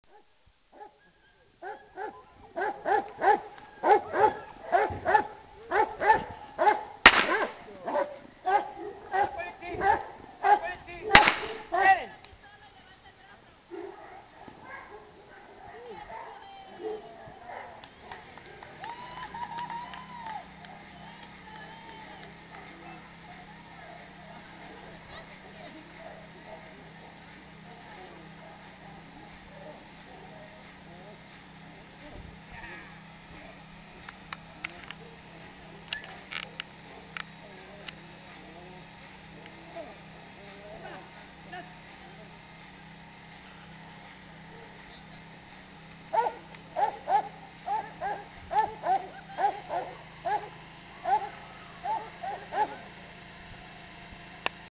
KNPV 2011 PIBE ARAGUA 2
En este video se muestra el ataque de un perro KNPV, sometiendo al agresor.